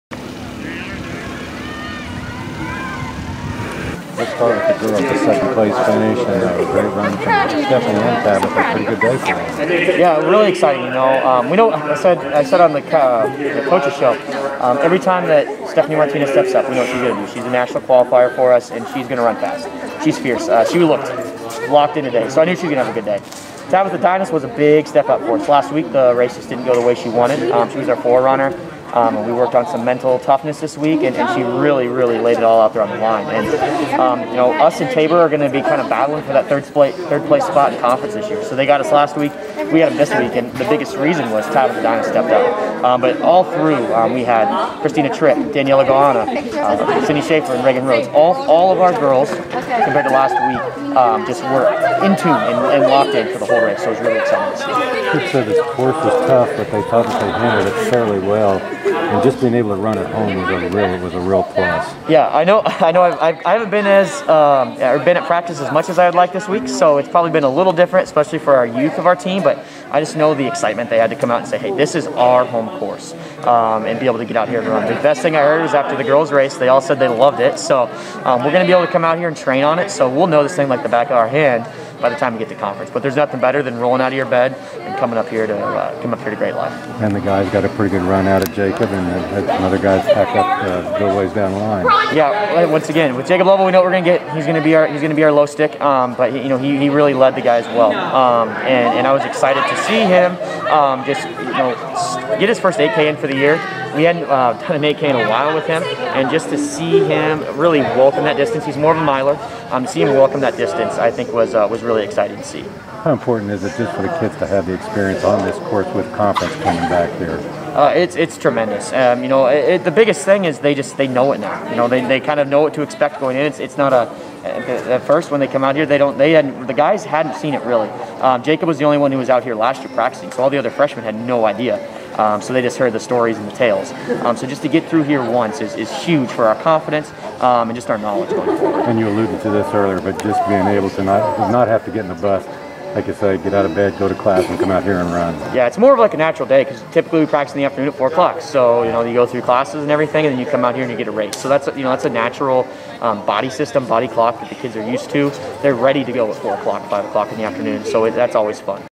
Cross Country Meet Highlights and Coach Hiser Interview
Check out some highlights from the KWU Cross Country meet on September 11, 2020, and an interview with Coach Kyle Hiser